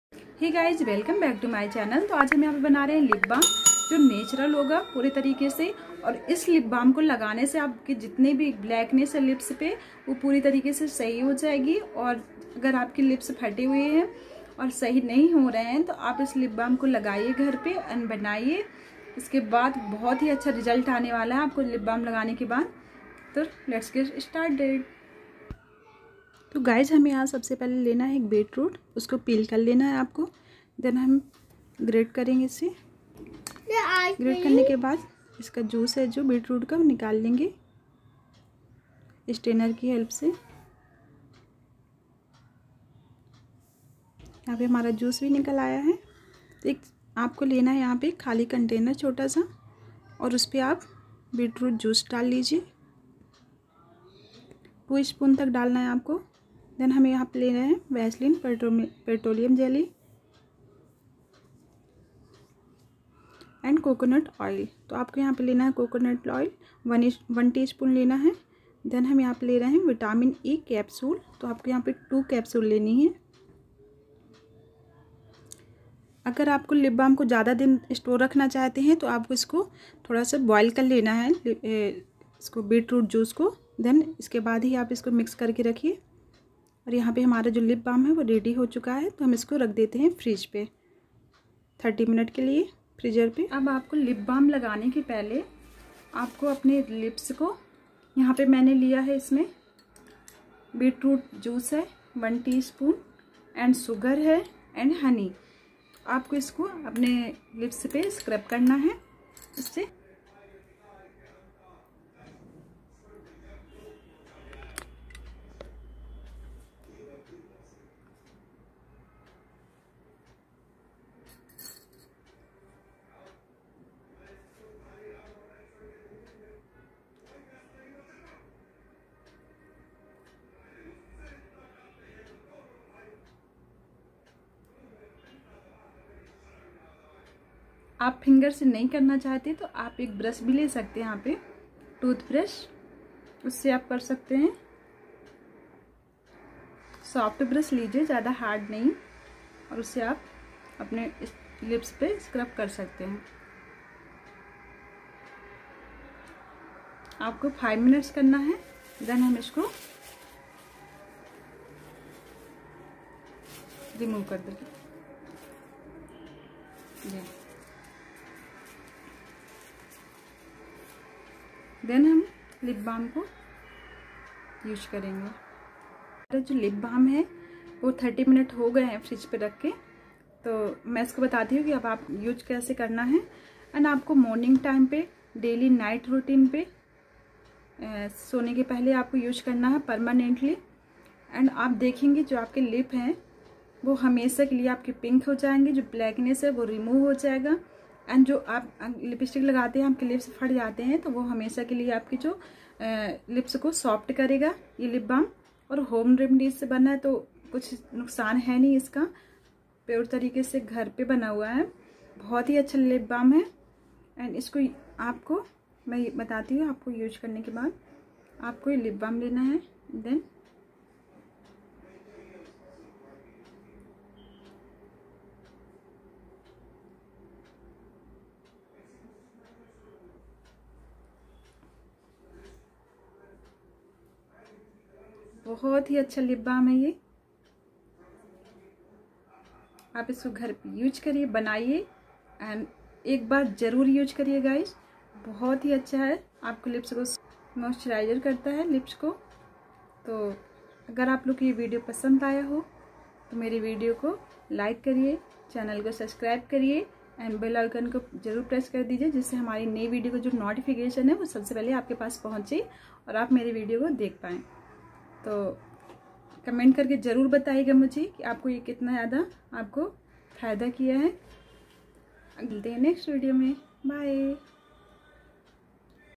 🍓🥕ऐसा Lipbalm जो आपके होठों को हमेशा के लिए गुलाबी बना देगा || permanent pinklips❤@PriyaDeepVlog
Homemade Lipbalm#nuturallipbalm
permanent pink lips lipbalm
100% result